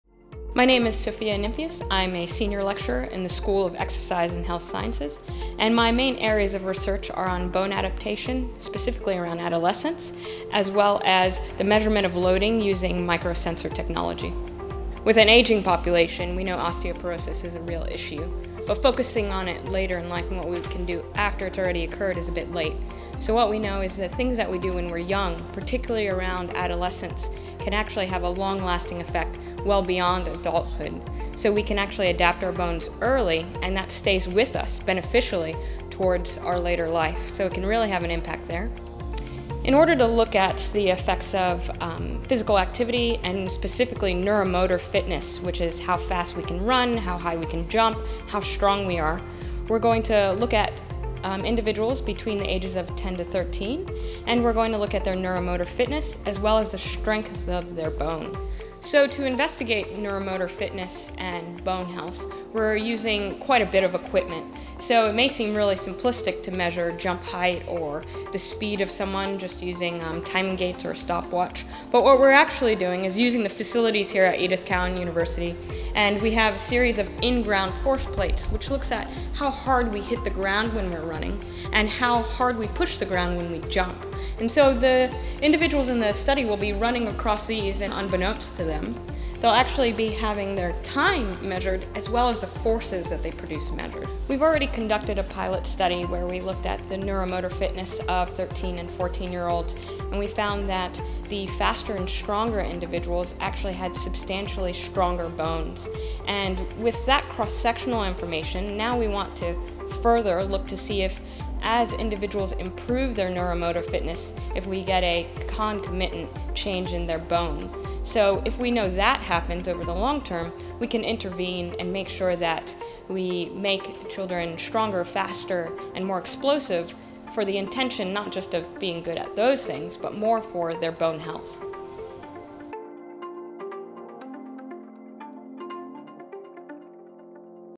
Assessment of changes in adolescent neuromotor fitness and bone strength: Dr Sophia Nimphius
Dr Sophia Nimphius, Senior Lecturer in the School of Exercise and Health Sciences at Edith Cowan University, discusses how certain activities that require rapid movement or use of our muscular strength, when performed during or prior to adolescence can influence the shape and strength of our bones and these changes may have a lasting effect!

Lifelong bone health of individuals is critically determined by our bone development during adolescence when we lay down the skeletal foundation that is protective and persists in our bone health for our entire lifetime. Therefore, the information gathered from the current project will provide specific understandings of the underlying physical fitness attributes that contribute to the development of bone strength during adolescence. As declining bone mass and architecture in later life is a major cause of morbidity and mortality, strategies to prevent or delay such issues are very important to the health and wellness of the population.

Our initial cross-sectional research has already provided extraordinarily positive support for the idea that activities like sprinting, jumping and improving or muscular strength directly influences the “shaping of our bone”, allowing for a “stronger and better built bone” when performed during those critical foundation years of bone growth around adolescence.

The proposed project has implications for future recommendations on activities to improve bone strength during adolescence by placing even greater emphasis on neuromotor fitness during adolescence. Overall, this research is a critical preliminary step in a series of studies required to determine the most effective and efficient ways to maximise our long-term bone health.